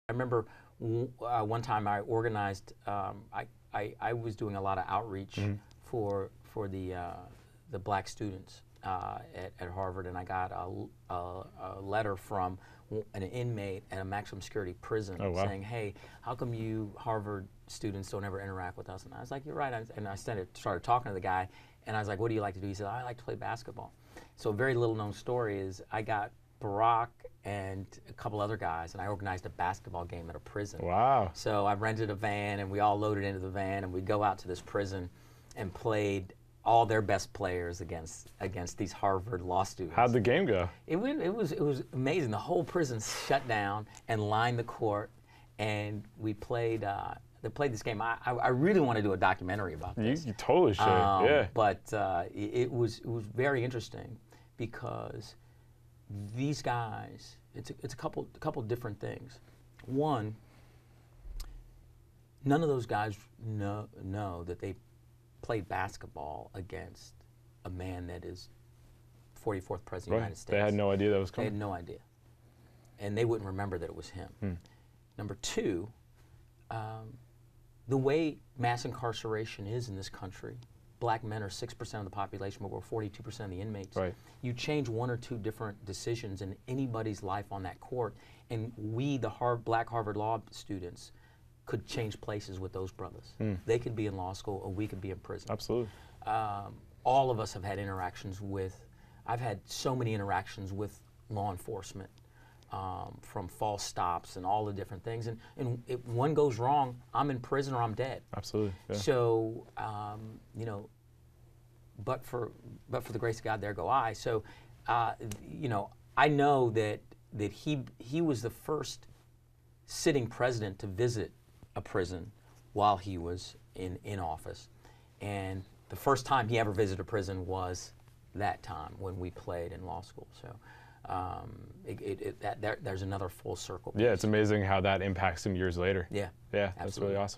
Actor Hill Harper Discusses Playing Basketball With Barack Obama At Harvard
Harper, an actor and author, attended Harvard alongside former President Obama and organized a basketball game at a prison while they were both students.